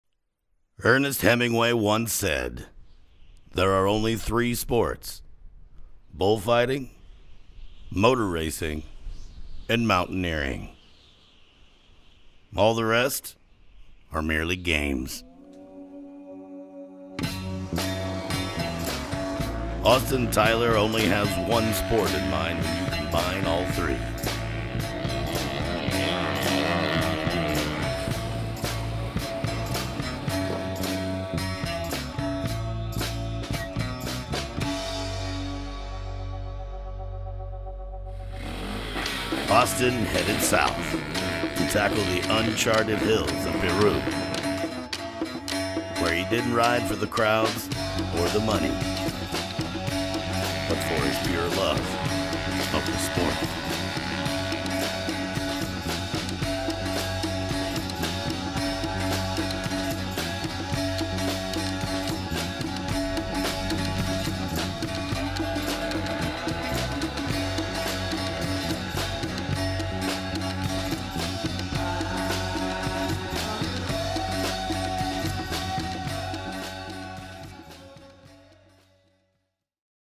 Uncharted: Superfly Climbs Peru (Official Trailer)
Last year, I went down to Peru with Elevated Action Sports and filmer Steve Halama, to climb some big hills! I packed some IRC Tires and swingarm extensions to convert the Socopur Motorsports KTM 450 SX-F over to a climber, and traveled the country for a week finding new hills. This trailer only shows a small portion of the action we experienced while in Peru.

This trip couldn't have happened without the help of Branko Bozovich, Ramiro Barco, and Elvis Nacion!

Socopur Motorsports
KTM Peru
Mitsubishi Peru
Mojo Motosport
FXR
Black Diamond MX
B
Nut Up Industries
Xtnded
BK8
El Huacachinero